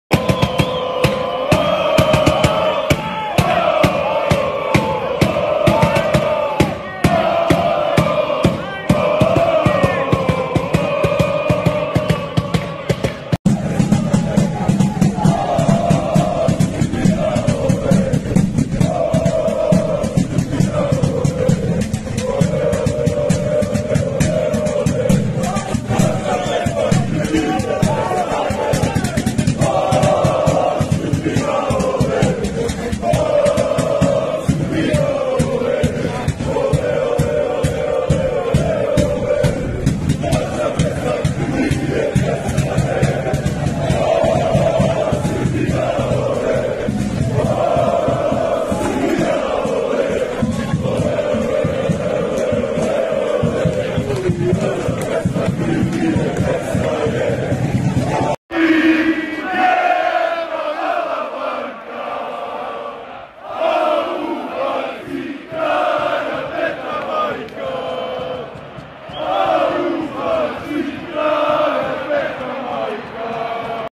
Pogledajte‼️ Korteo Srba u Podgorici pred meč Crna Gora-Srbija serbs montenegro serbia fans 🇷🇸
crna gora srbija